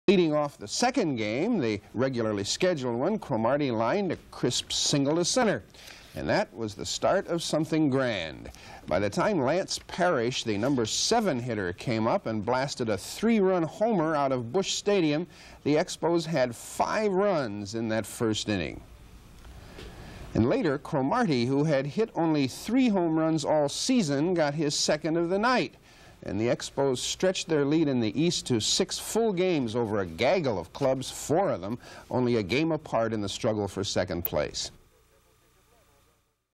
1979 06/25 Expos 8 at St. Louis 2
Larry Parrish hit a 3-run home run that helped the Expos to a 5-0 first inning lead. Warren Cromartie added a home run of his own and the Expos downed the Cardinals 8-2 in St. Louis on June 25, 1979.